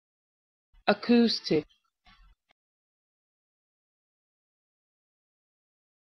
How to Pronounce Acoustic
This video shows you how to pronounce Acoustic